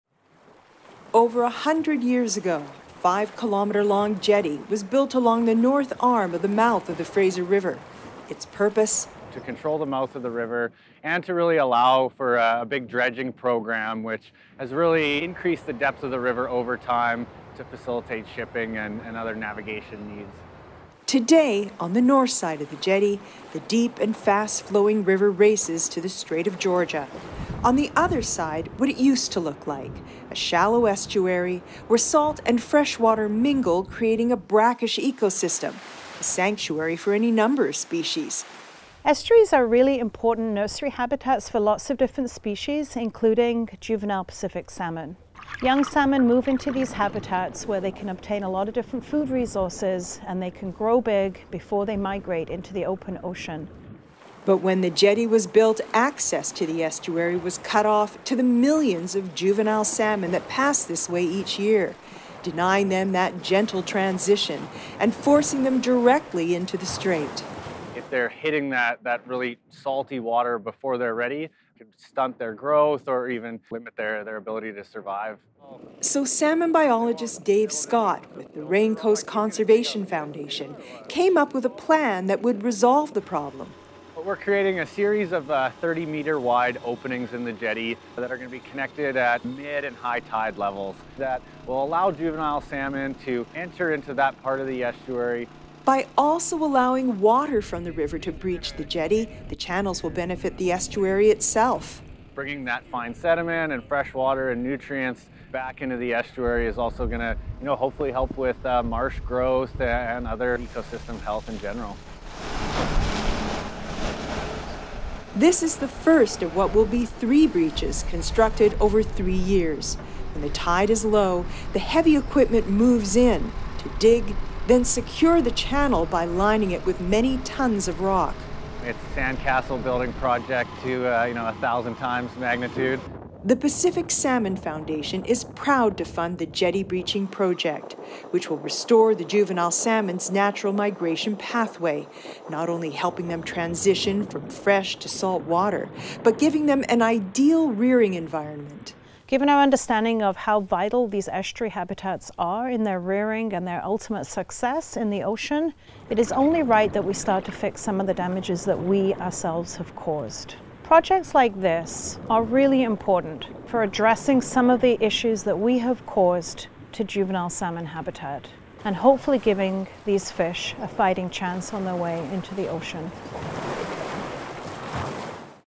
Immediate success for salmon habitat restoration in the Fraser River
Cut off from their essential habitat for more than 100 years, threatened Fraser Chinook salmon find new passage via large-scale estuary project.

The North Arm of the Fraser River Jetty habitat restoration project, estimated at $650,000, provides a unique solution to a century-old problem. The jetty, a 7-kilometre-long barrier, has been forcing young salmon from the freshwater of the Fraser River’s North Arm into the saltier, deeper water of the Strait of Georgia for more than 100 years. As juvenile salmon rely on the brackish marsh habitats that lie in between the river and the Strait of Georgia to rear and feed before they transition to the ocean, the jetty presents a serious barrier to their well being.

“Estuaries are really important nursery habitats for many species including juvenile Pacific salmon,” says Dr. Isobel Pearsall, director of PSF’s Marine Science program. “This environment allows them to obtain rich food resources so they can grow bigger before they migrate into the open ocean.”

As part of the project, Raincoast Conservation Foundation made a deliberate breach in the North Arm jetty in early March; they have been monitoring the 30-metre wide opening, and are already observing juvenile salmon passing through.

This immediate success makes this habitat restoration project rare.

The project is led by the Raincoast Conservation Foundation and its partners, including the Pacific Salmon Foundation (PSF), Ducks Unlimited, Tsawwassen First Nation, and Lower Fraser Fisheries Alliance.
Harrison River Chinook used to be the most productive Chinook salmon population in the Fraser, but has been declining for more than a decade and is now considered threatened by the Committee on the Status of Endangered Wildlife in Canada (COSEWIC). Juvenile Chinook, likely from the Harrison River, have been captured using the breach passage during spring 2022.
“Juvenile salmon rely on access to estuary habitats to adapt to salty waters and for important feeding opportunities that can improve their ability to survive in the ocean,” says Dave Scott, a biologist with Raincoast and a PhD-candidate in the Pacific Salmon Ecology and Conservation Lab at UBC.

The Lower Fraser River and estuary is a highly modified environment with more than 80% of tidal marsh habitats, which juvenile salmon rely on, either lost or inaccessible. On the Fraser delta, this includes the jetties, causeways, and training walls that were built to control the arms of the river for ship navigation.

This restoration follows on Raincoast’s success creating three breaches in the Fraser River’s South Arm on the Steveston Jetty in 2019. Raincoast biologists found high numbers of juvenile salmon passing through to reach the marsh on Sturgeon Bank, instead of being swept out to the Georgia Strait.
Raincoast plans on creating two more 30-metre-wide breaches in the North Arm Jetty to continue to restore natural migration pathways for juvenile salmon, other fish species, and natural flow of freshwater and fine sediments.
“Given our understanding of how vital estuaries are, it is only right that we start to fix some of the damage we humans have caused. Projects like this are really important to address some of the issues we have caused to juvenile salmon habitat…and hopefully give these fish a fighting chance on their way into the open ocean,” says Dr. Pearsall.

This project was made possible by funding from the Pacific Salmon Foundation, the Province of BC and the Government of Canada.